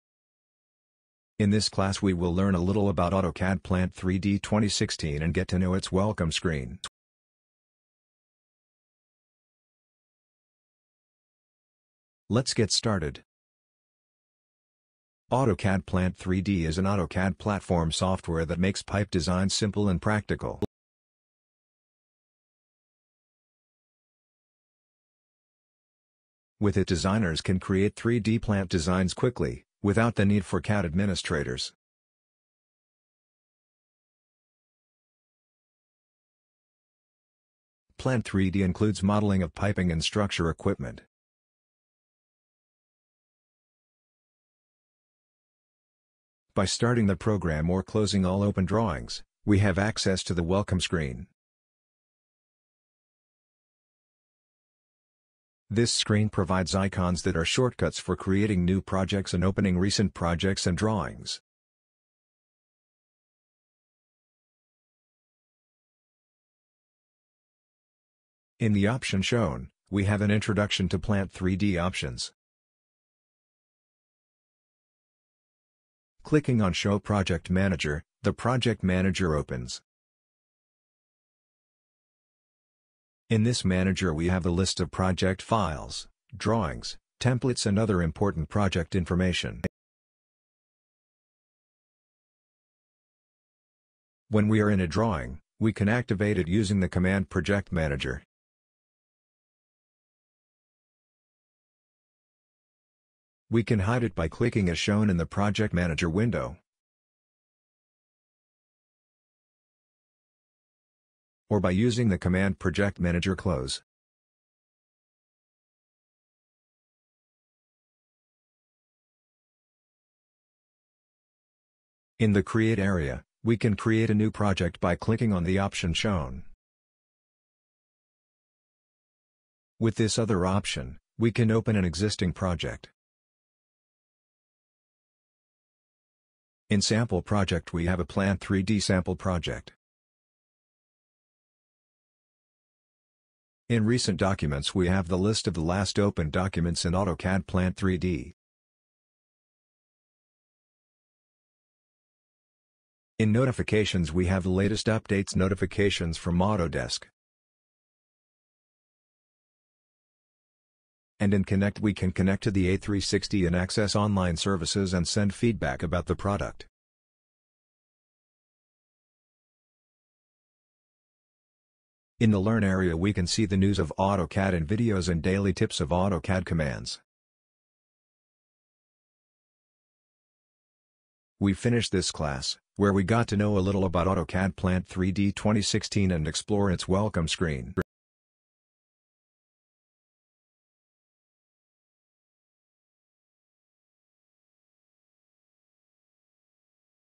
2 - Getting to know the Program and its Start Screen | AutoCAD Plant 3D 2016 Structural Modeling
In this class we will learn a little about AutoCAD Plant 3D 2016 and get to know your welcome screen.